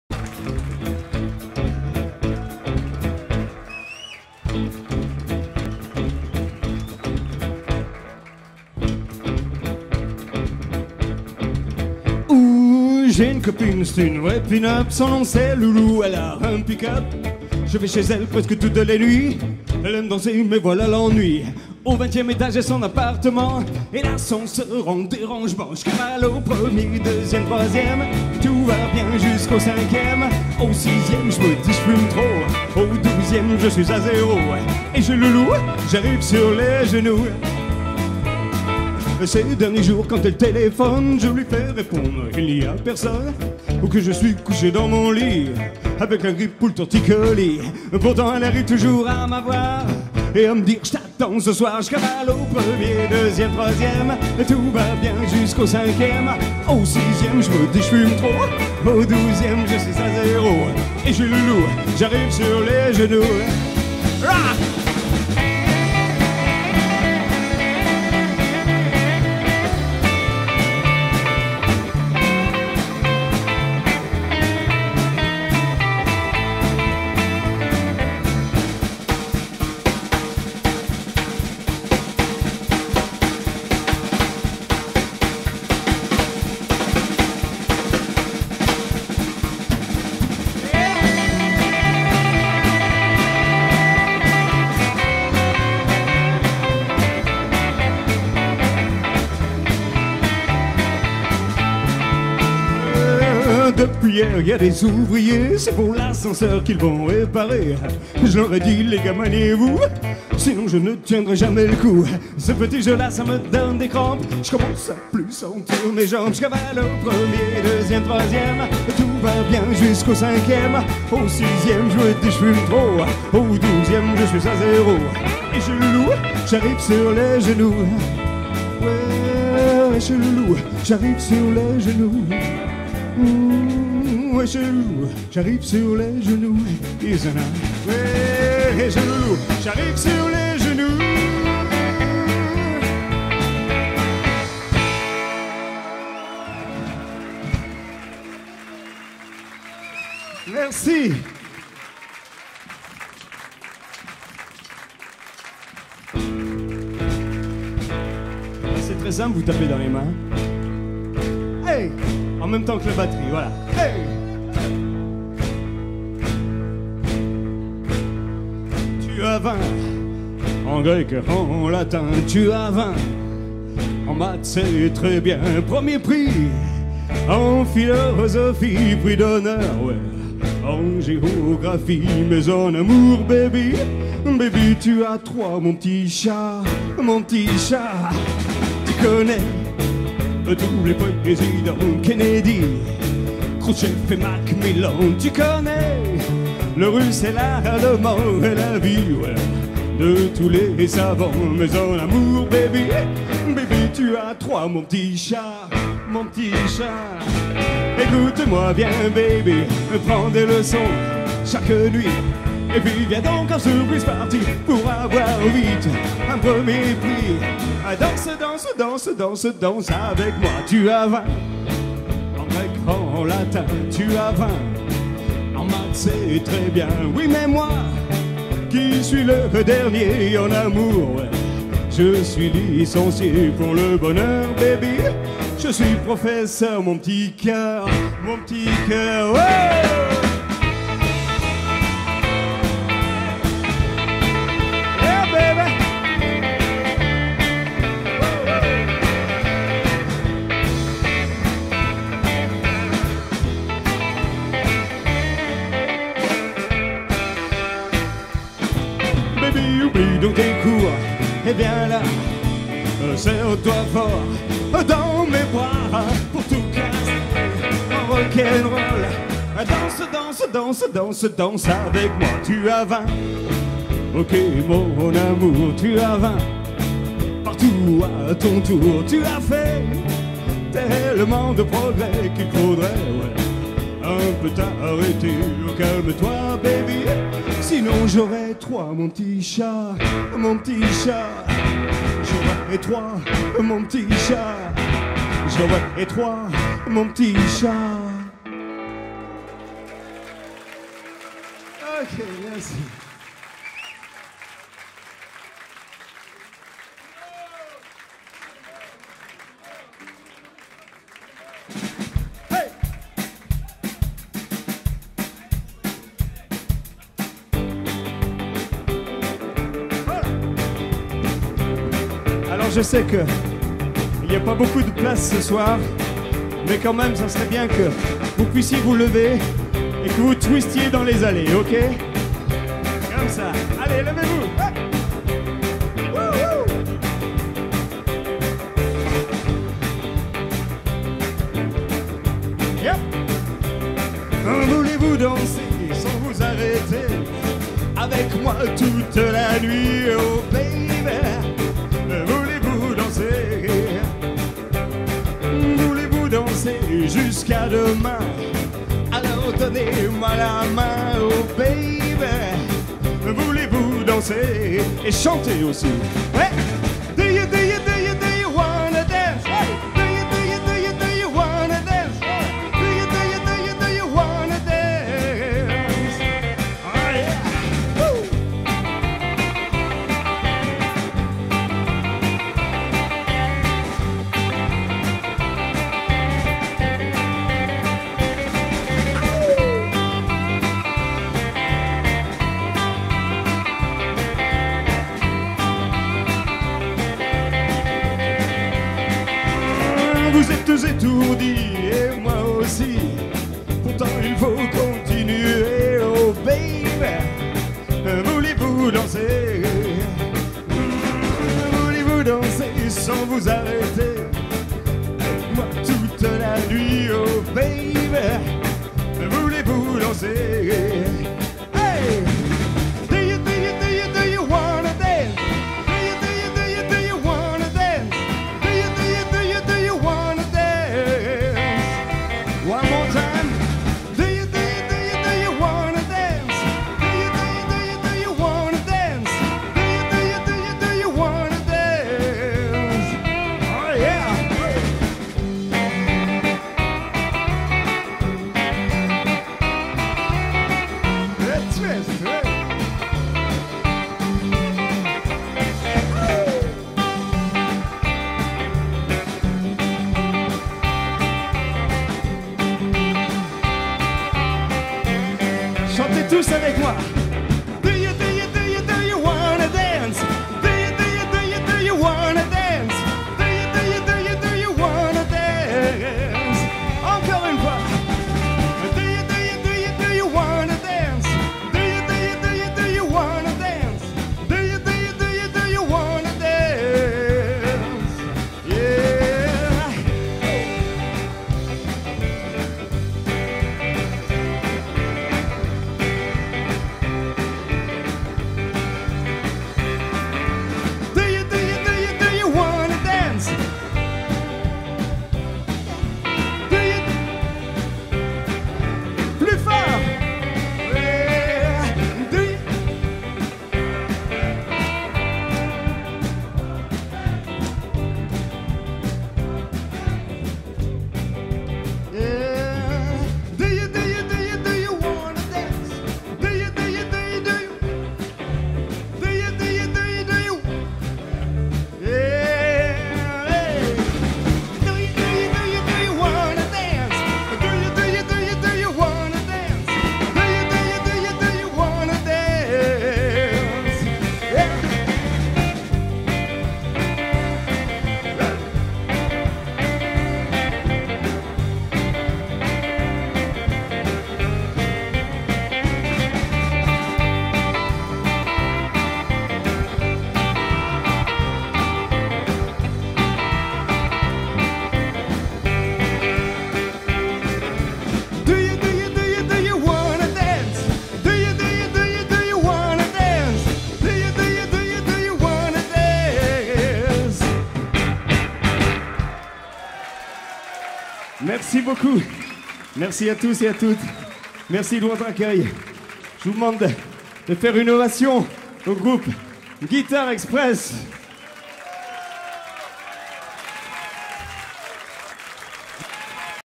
MON AMI CHRIS EVANS ET LES GUITARES EXPRESS ROLLMOPS
MON AMI CHRIS EVANS ET LES GUITARES EXPRESS AU PETIT JOURNAL MONTPARNASSE PARIS DU 26 OCTOBRE 2004 A++ROLLMOPS